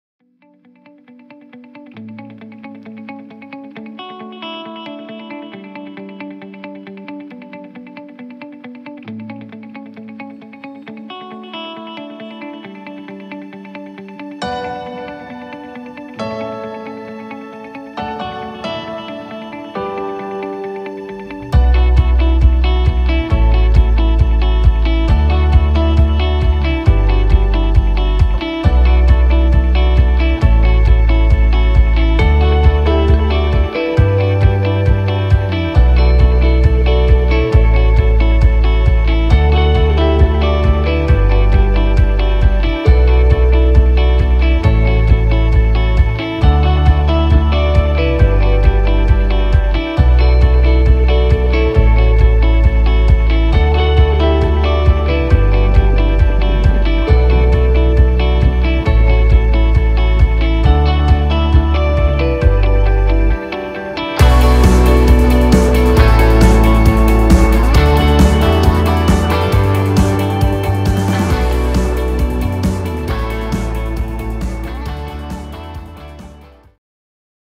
Cub Whirligig airplane from wood
Build of a Cub Whirligig from Wood.
Taken from plans for an FT Simple Cub